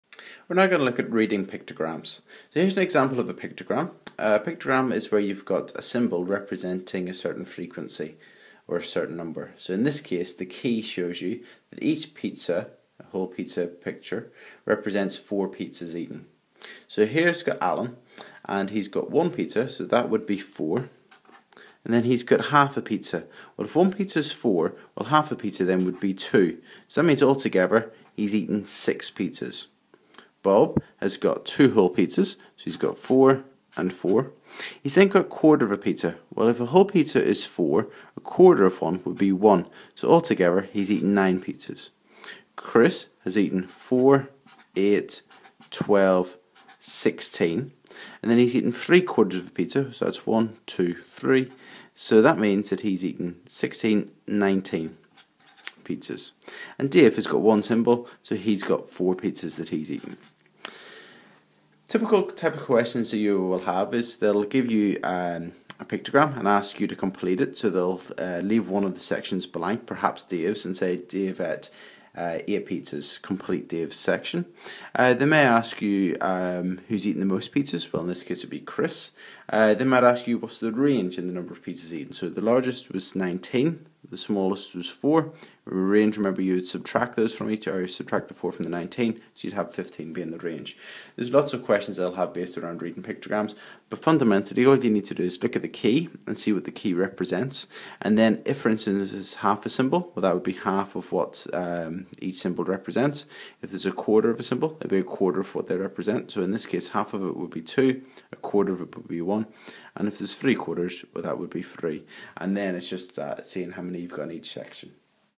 Reading Pictograms - Corbettmaths
Corbettmaths - A video that shows how to interpret pictograms.